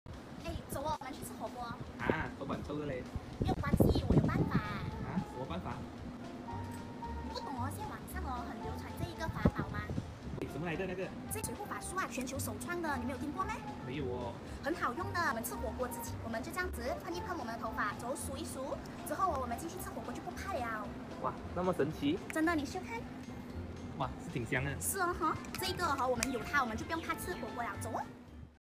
D’COEUR is the secret weapon to assure a wonderful steamboat date
When her boyfriend is reluctant to go for steamboat because the hair will smell bad, check out how this confident and gorgeous lady whips out a secret weapon to reassure him.

D’COEUR, The World’s First Hair Perfume Treatment is absolutely a treasure you can get online to convince not just your boyfriend, but everyone that they can enjoy their steamboat date, and still smell lovely.